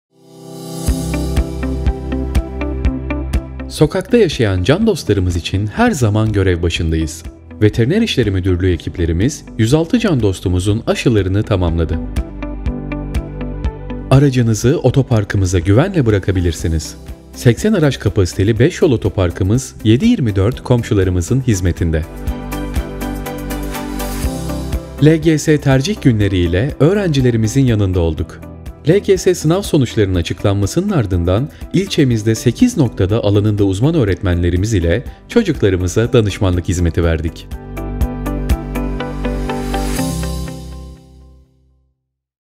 Küçükçekmece'de HaftanınAdımları 18 - 25 Temmuz
📌 Sokakta yaşayan can dostlarımız için her zaman görev başındayız. Veteriner İşleri Müdürlüğü ekiplerimiz 106 can dostumuzun aşılarını tamamladı.
📌 Aracınızı otoparkımıza güvenle bırakabilirsiniz. 80 araç kapasiteli Beşyol Otoparkımız 7/24 komşularımızın hizmetinde.
📌 LGS Tercih Günleri ile öğrencilerimizin yanında olduk. LGS sınav sonuçlarının açıklanmasının ardından ilçemizde 8 noktada alanında uzman öğretmenlerimiz ile çocuklarımıza danışmanlık hizmeti verdik.